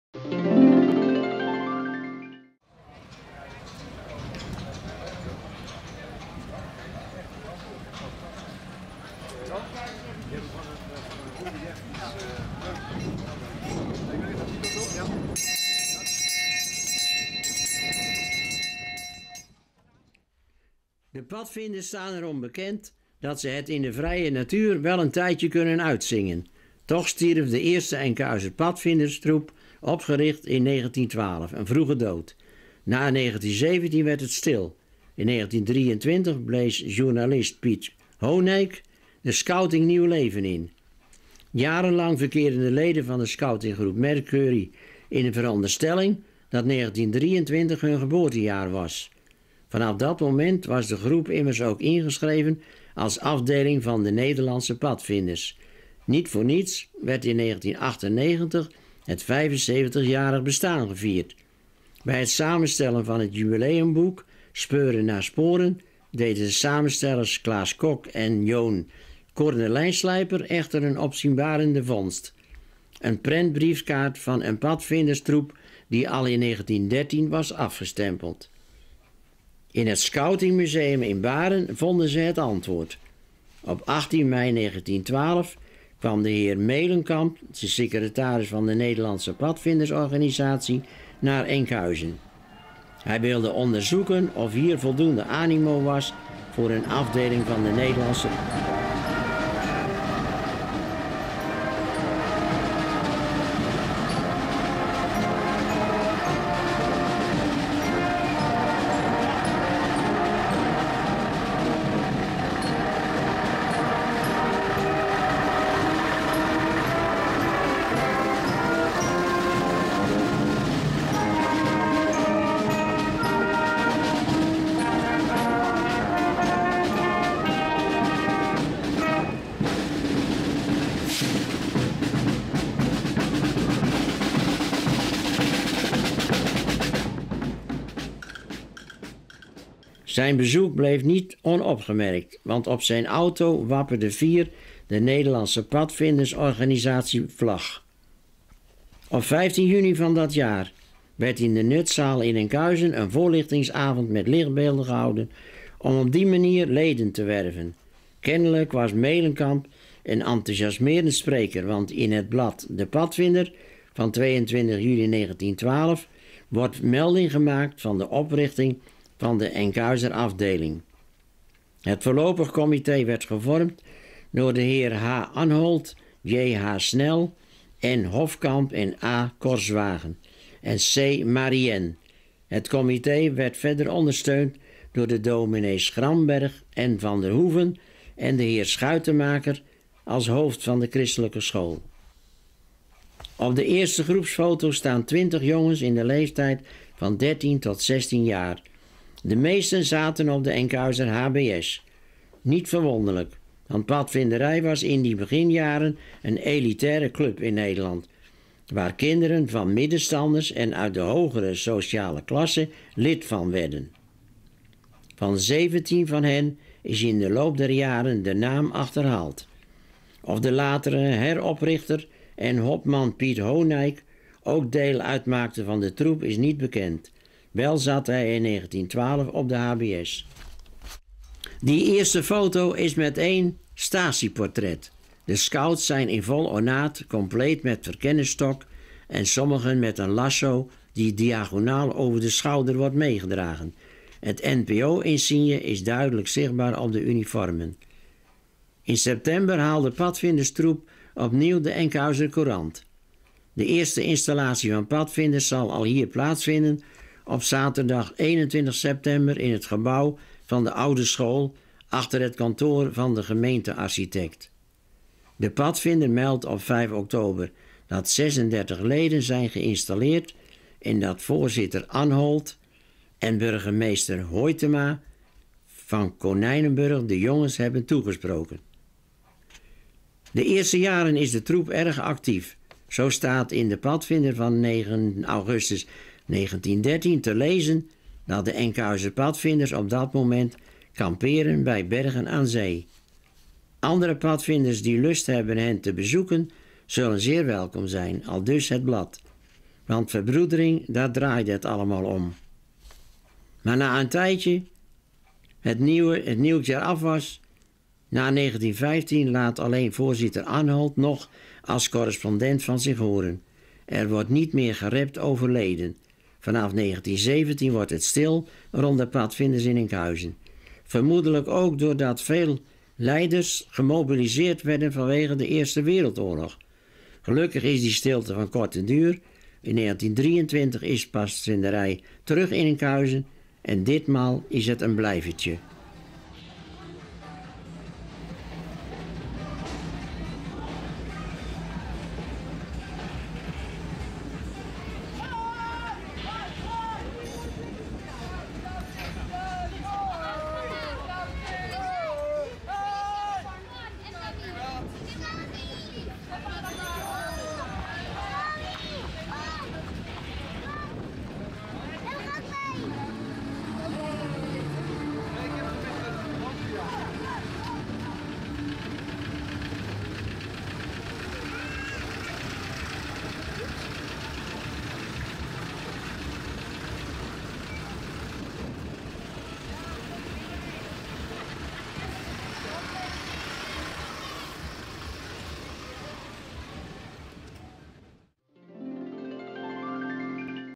100jaar scouting Enkhuizen 2012.
Dit  jaar 2012 is het 100jaar bestaan van de Scouting uit Enkhuizen